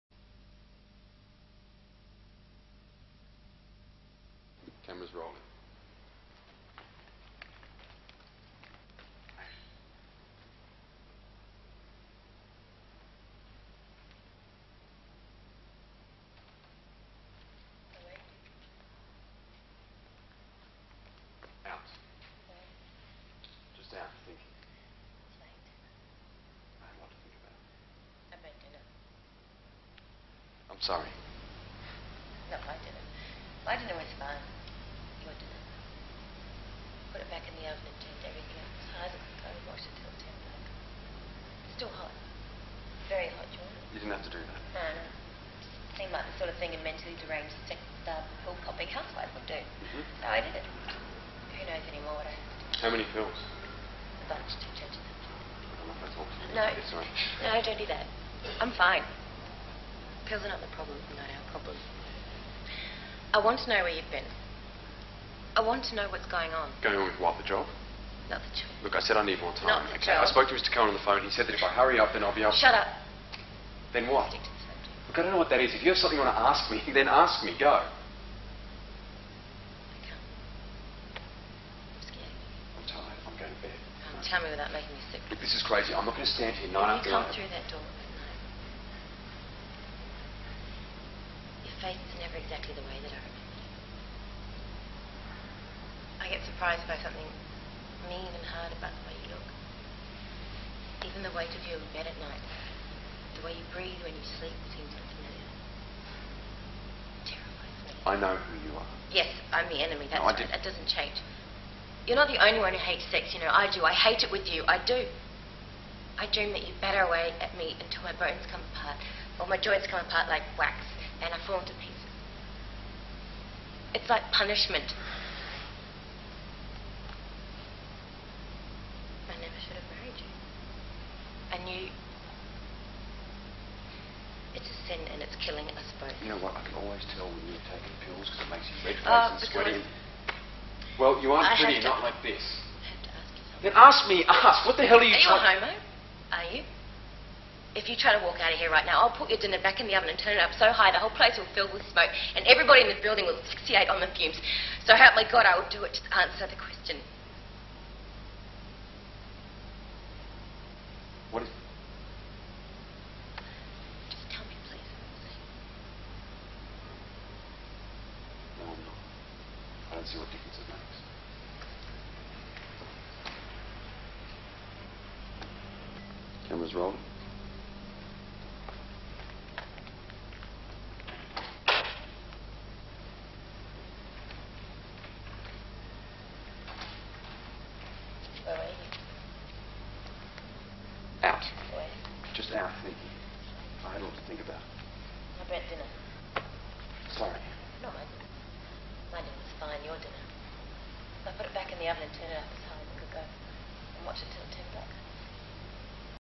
Nic Bishop and Vanessa rehearsing 2009
I am so grateful for my teachers who have helped me along my path, the brilliant Nic Bishop was my coach in 2009 and here are some clips we saved.